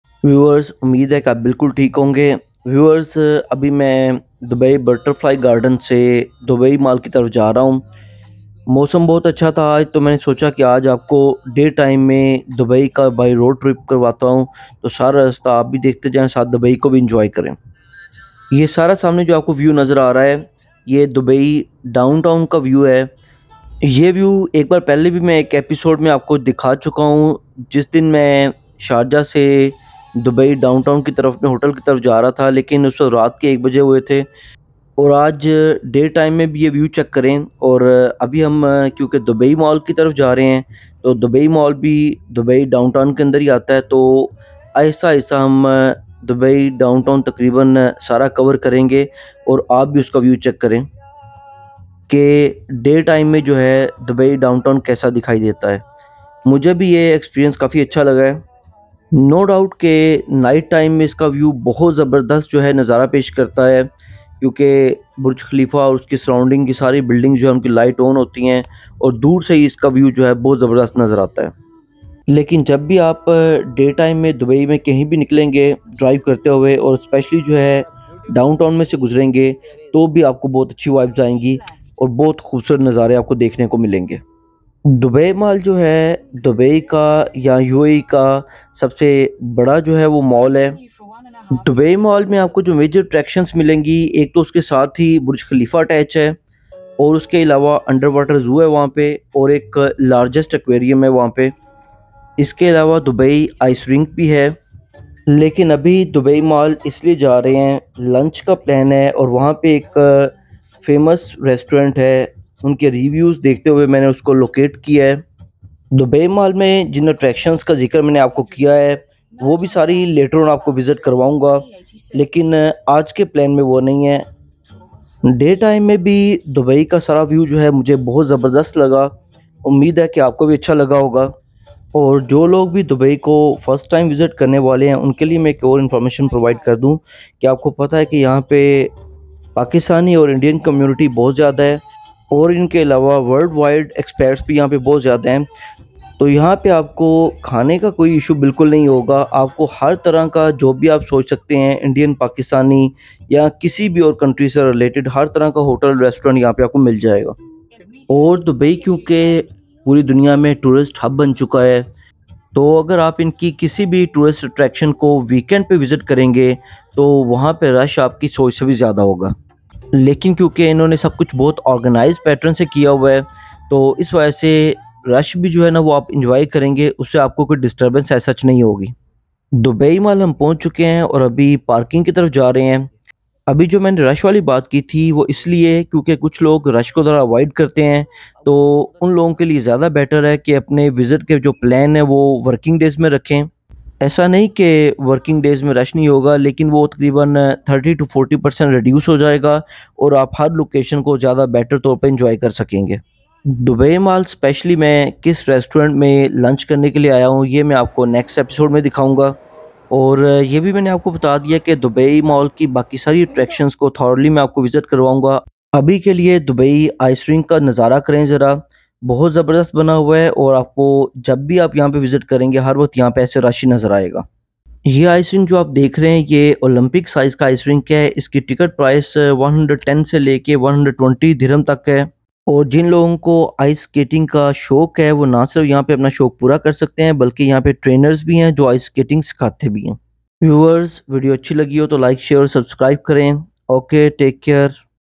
A Road Trip Of Dubai| Dubai Ice Rink|Skyscrapers| Dubai Tour/Season Episode 10
Hello viewers,

i.How to plan a visit of dubai
ii. A road trip of dubai
iii. Dubai mall
iv. Dubai ice rink
v. Life in dubai